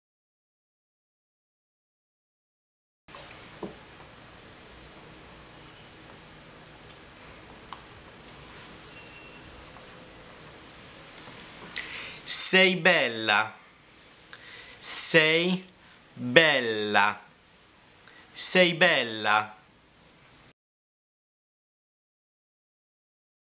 重返古羅馬 展覽×樂園》義大利單字教學－第四課
義大利語的「妳好漂亮」怎麼說？
女孩們到了義大利最常聽見的一定是：Sei Bella
Sei Bella! Sei Bella! Sei Bella!
學習了嗎？下次聽見這句話，就回報一個微笑吧！